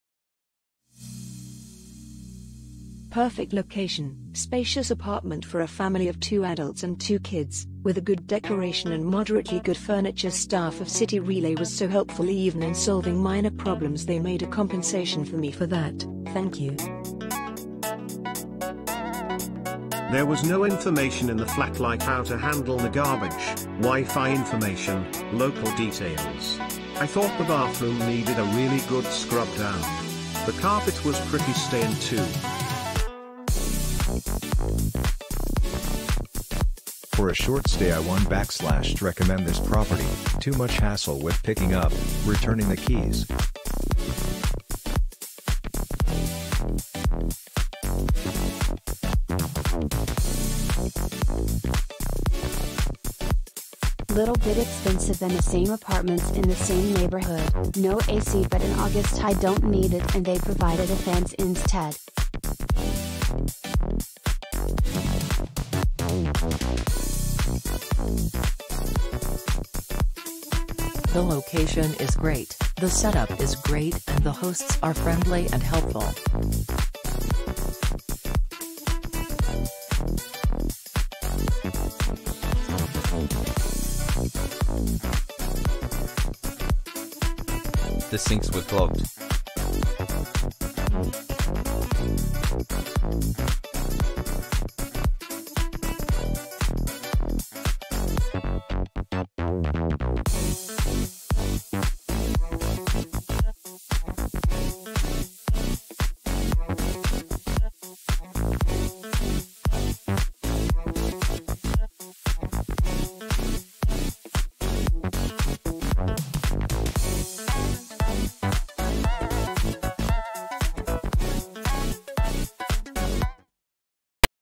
The Stratford Road Pied-a-terre | Reviews real guests Hotels in London, Great Britain
One of our best sellers in London! The Stratford Road Pied-a-terre is located in the Kensington and Chelsea district of London, 1.4 km from Natural History Museum London and 1.4 km from Olympia Exhibition Centre. Complimentary WiFi is available. The apartment includes 1 bedroom and a living room with a TV. A dishwasher, an oven and a toaster can be found in the kitchen. Victoria and Albert Museum is 1.8 km from the apartment, while Royal Albert Hall is 1.9 km from the property. The nearest airport is London City Airport, 19.3 km from the property.